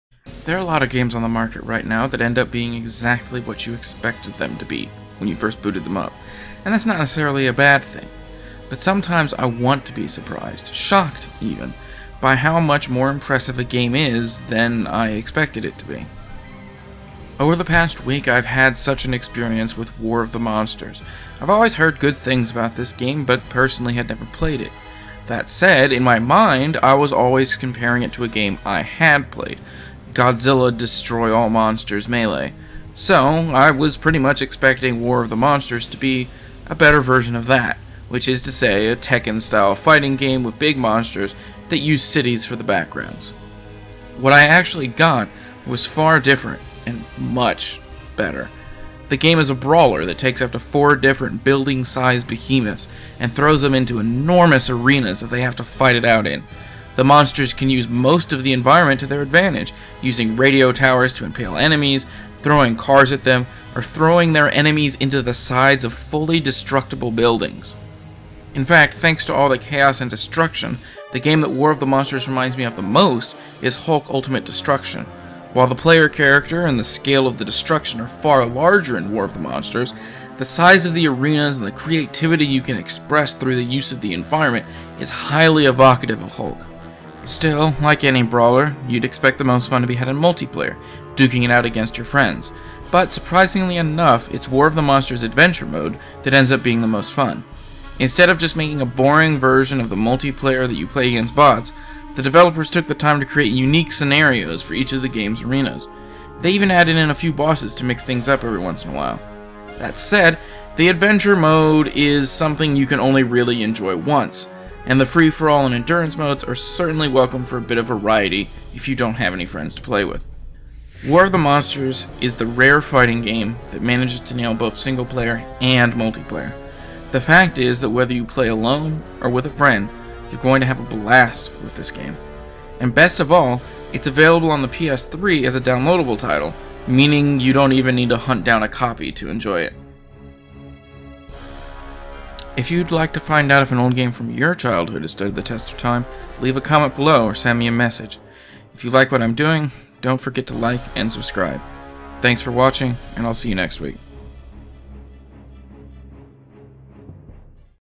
War of the Monsters: Retro Review
This kaiju fighting game has a legion of fans, but is it actually fun to play?